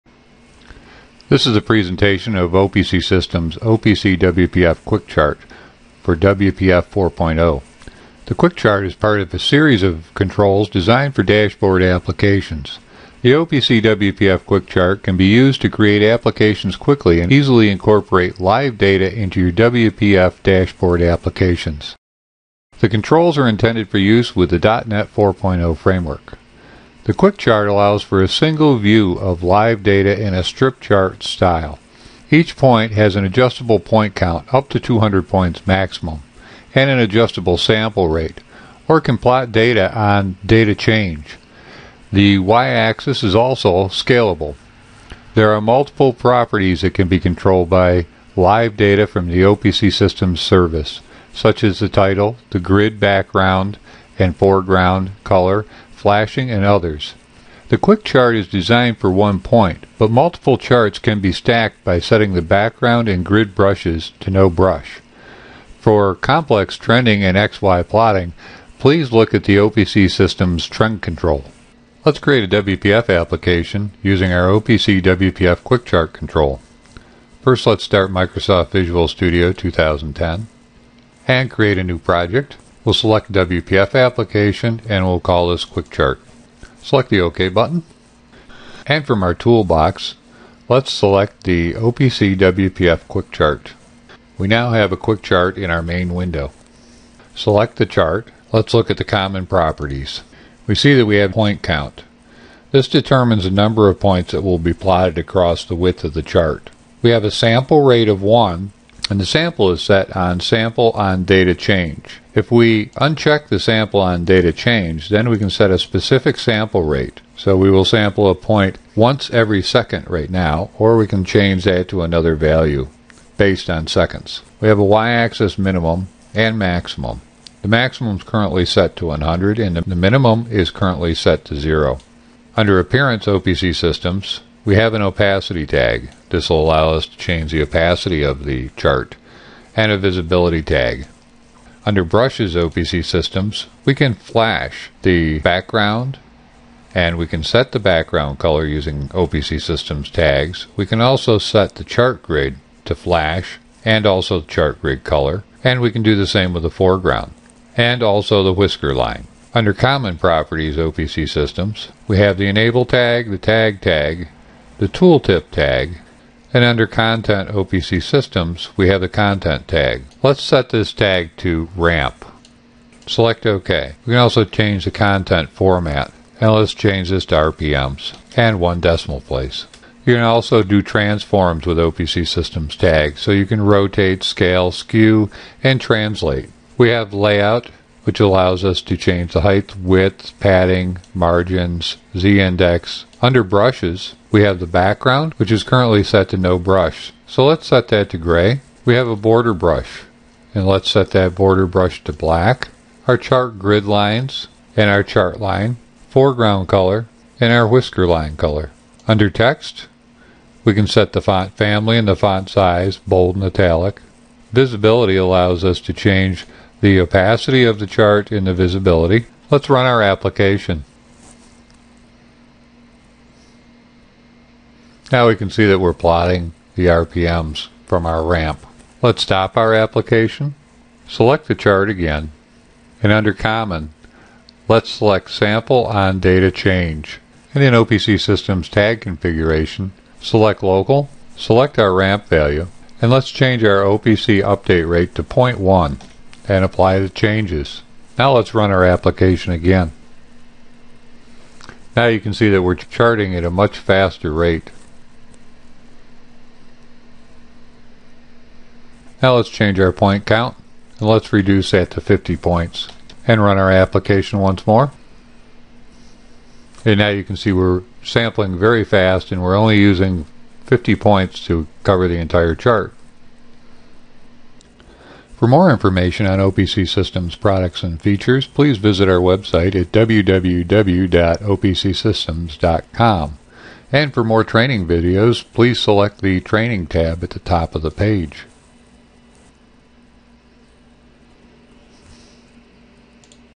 HMI Dashboard Quick Chart for WPF Applications
Learn how to implement the OPCWPFHMI.NET Quick Chart control with Visual Studio and Expression Blend 4.0.
OUTLINE
0:00 – Introduction
0:08 – OPC WPF Quick Chart for WPF 4.0
1:12 – Create a WPF Application using OPC WPF Quick Chart
1:39 – Common Properties
2:12 – Appearance OPC Systems
2:22 – Brushes OPC Systems
2:46 – Content OPC Systems
3:01 – Transform OPC Systems
3:29 – Text
3:40 – Run the Application
4:19 – Change the Point Count
4:35 – More Information